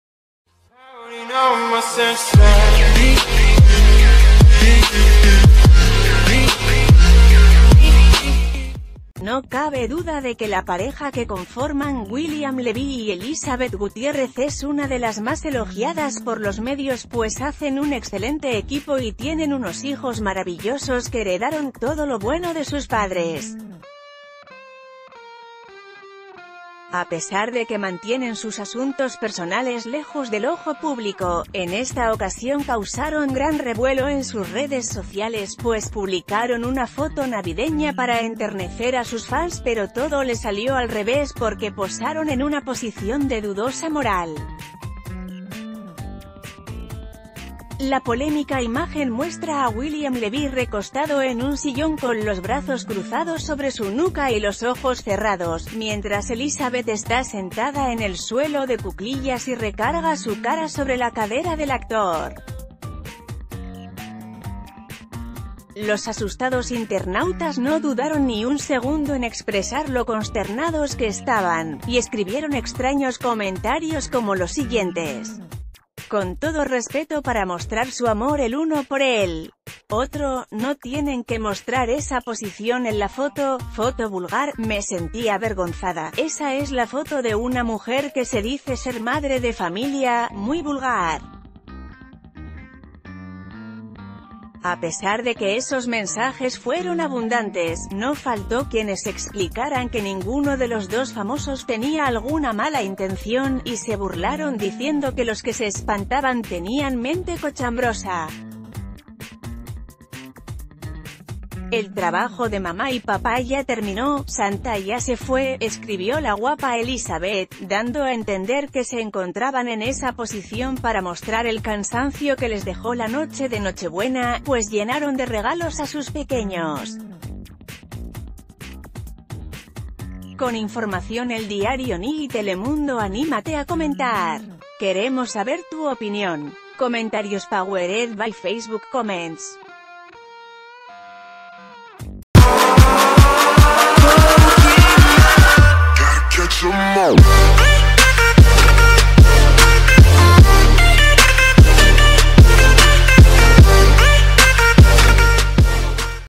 Critican a la esposa de William Levy por foto vulgar en el sofá
No cabe duda de que la pareja que conforman William Levy y Elizabeth Gutiérrez es una de las más elogiadas por los medios, pues hacen un excelente equipo y tienen unos hijos maravillosos que heredaron todo lo bueno de sus padres. A pesar de que mantienen sus asuntos personales lejos del ojo público,...